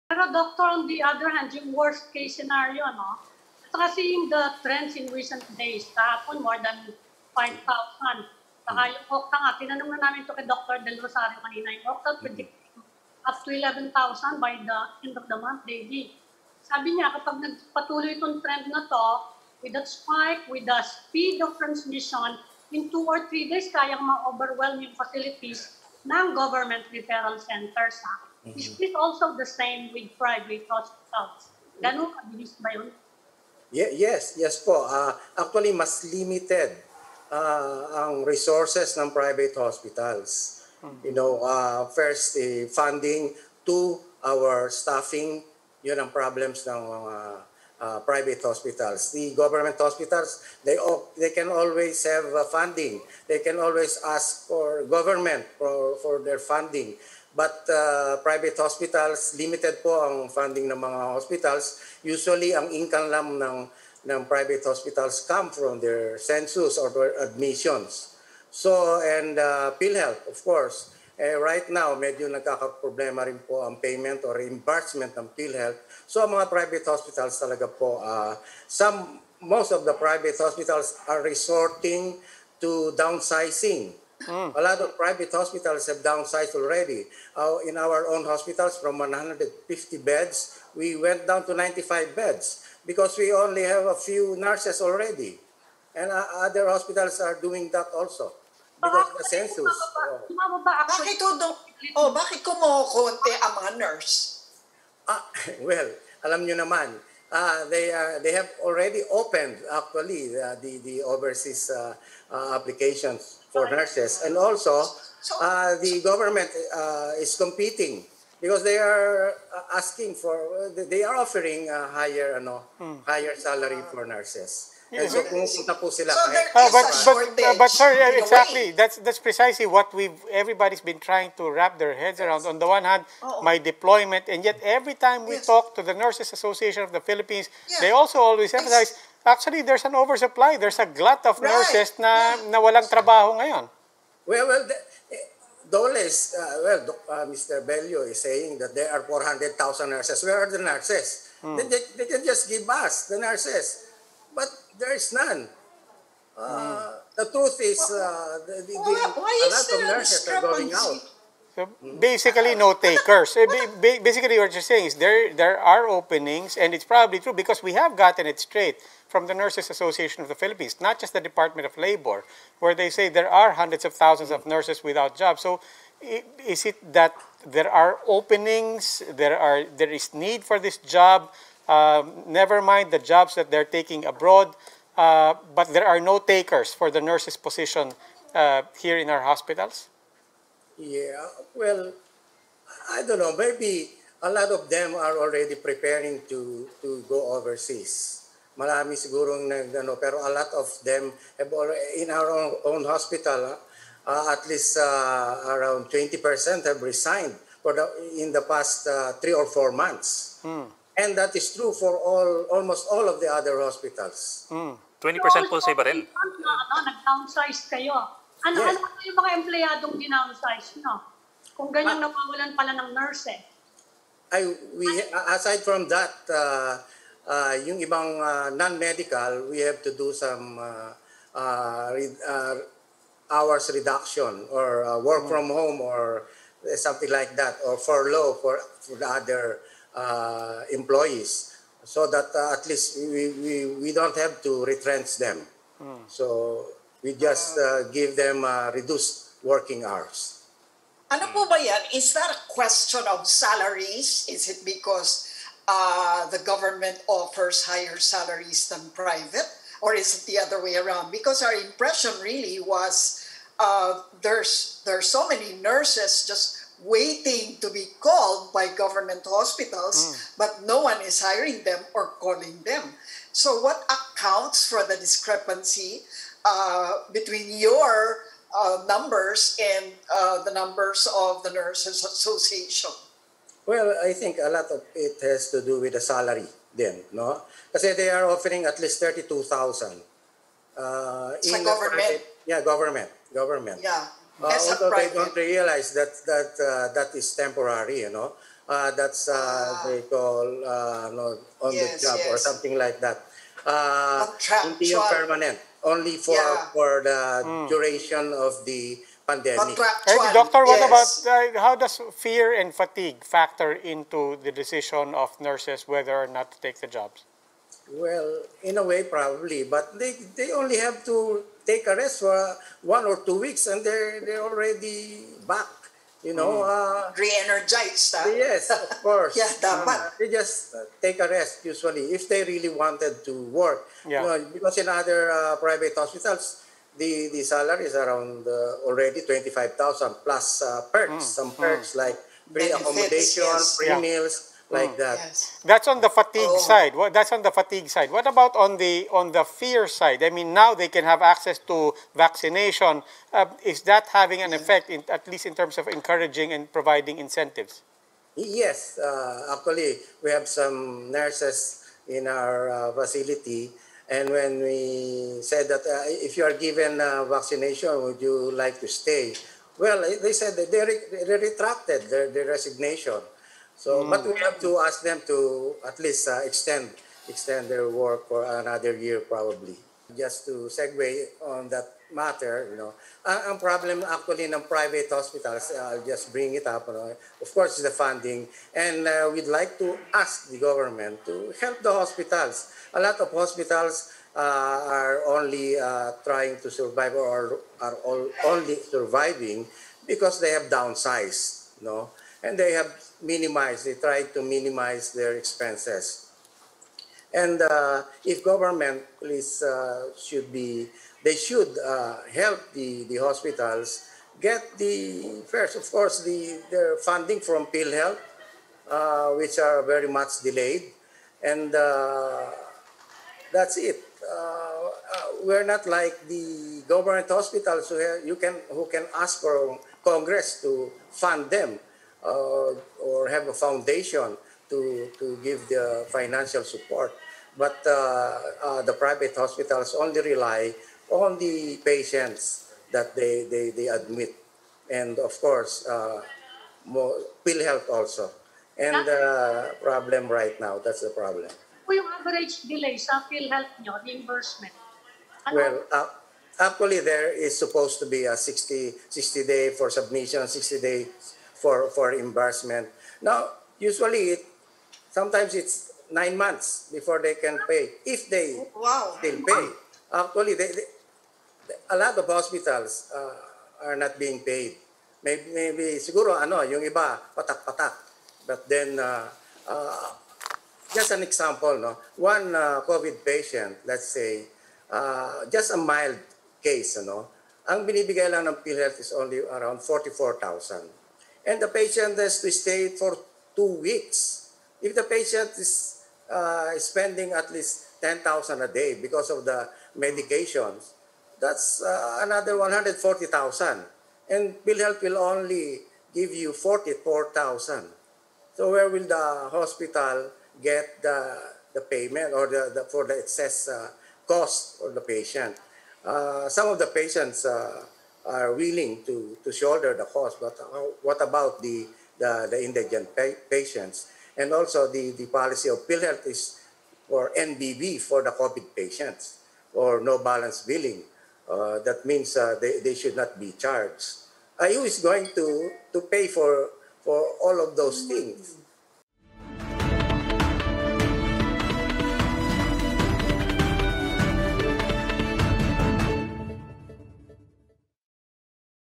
PHAPI seeks gov't help over delayed PhilHealth reimbursements
Dr. Jose de Grano of the Private Hospitals Association of the Philippines appeals for gov't aid, citing delayed reimbursements from PhilHealth and a shortage of nurses.